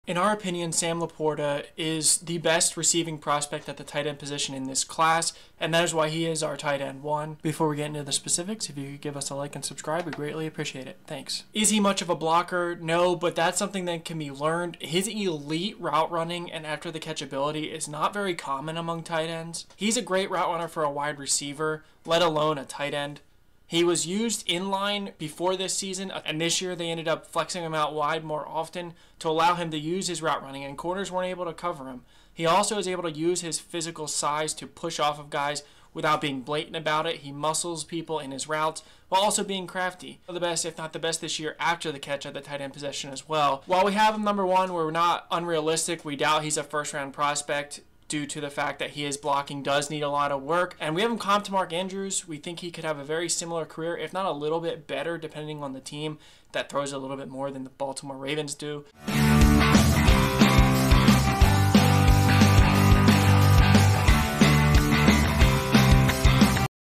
Sam LaPorta= Best TE Potential? | 2023 NFL Draft Prospect Profile
We discus why Sam LaPorta is our favorite tight end prospect and how his traits could make him the best tight end to come from the 2023 NFL Draft class

Credit to BIG 10 Network for Game Film we are not the owners